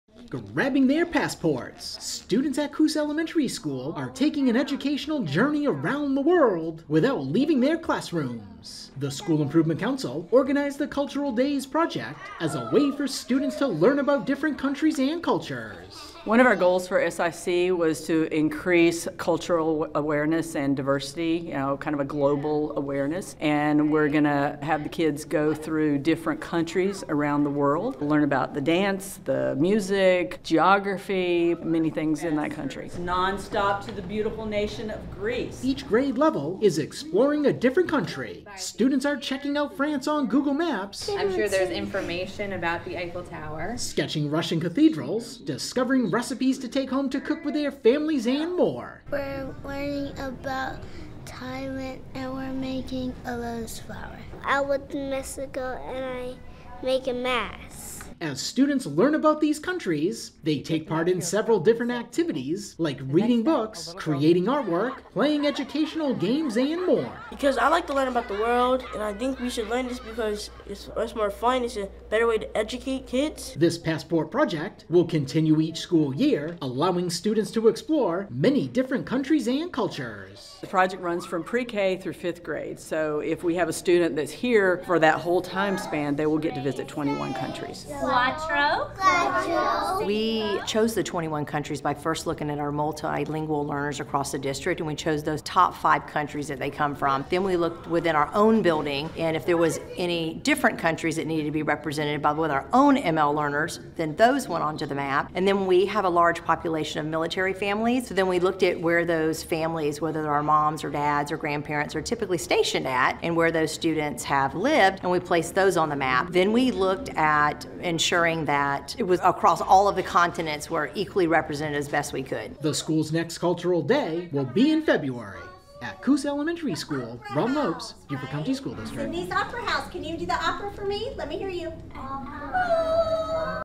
Passport Project | Cultural Day One 🌍✈️
Students at Coosa Elementary School are taking an educational journey by exploring several cultures from around the world – all without leaving their classrooms.